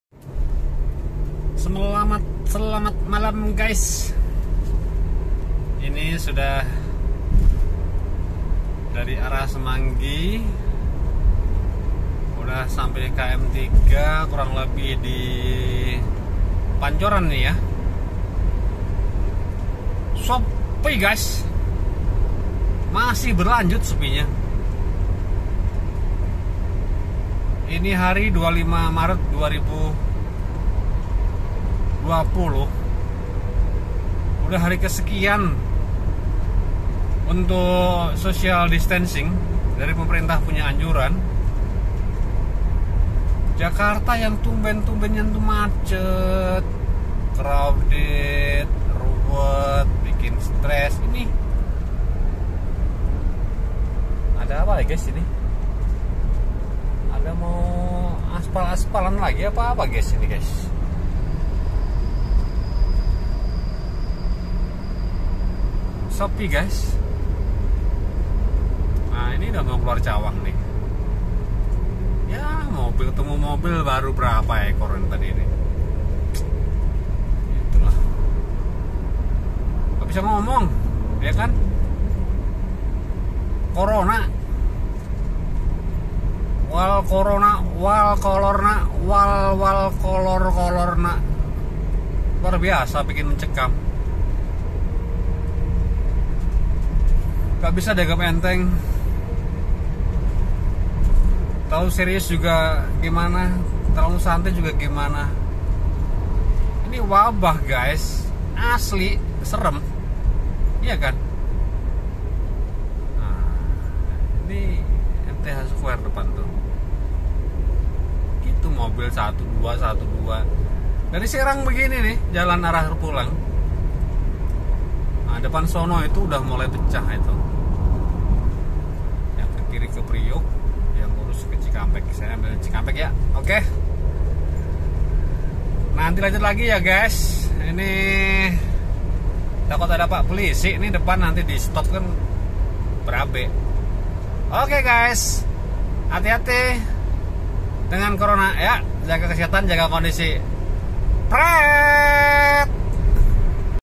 JAKARTA SUEEPI
Ibukota Jakarta kini tdk macet lagi. akibat sosial distancing berskala besar